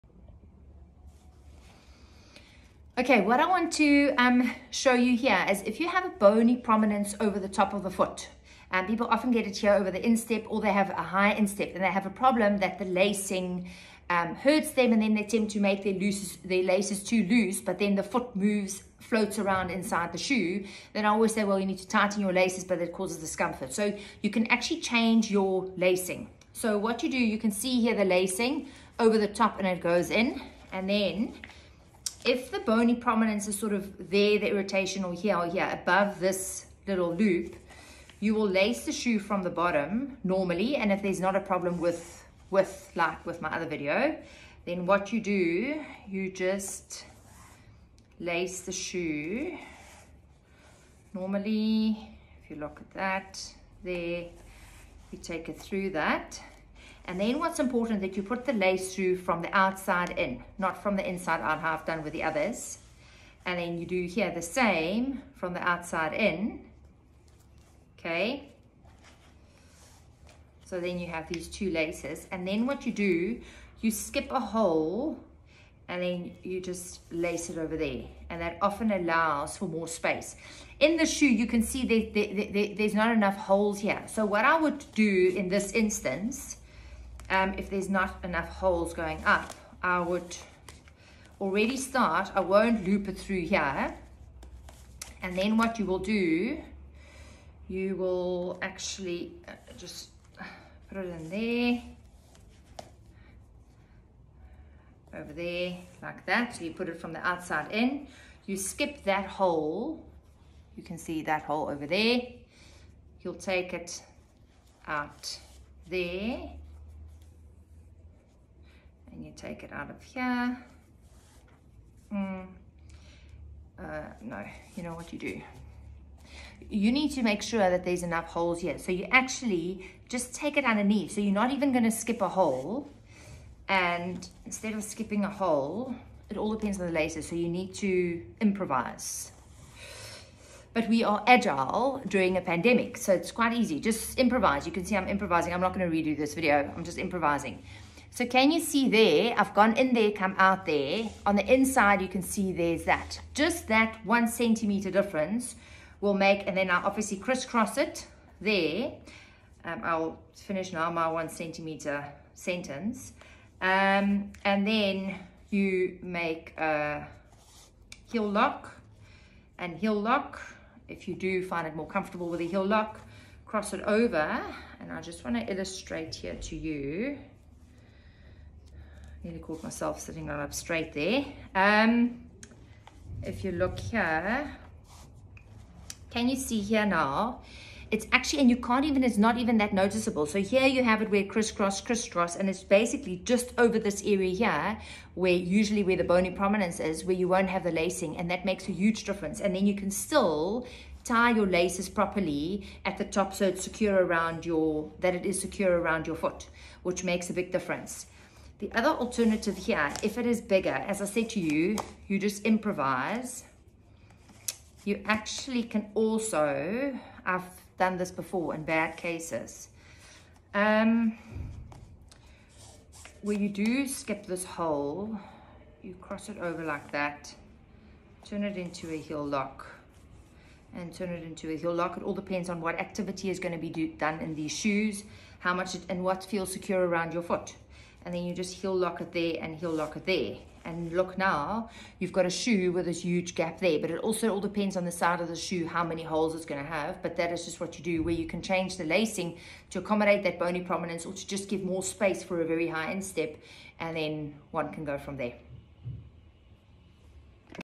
Lacing over bony prominences
Some people have trouble with bony prominences over the top of their feet/ foot and lacing often causes discomfort. Here is a suggestion on how to change your laces to "accomodate" the bony prominence and still have the secure lacing that allows your foot and shoe to function as a unit.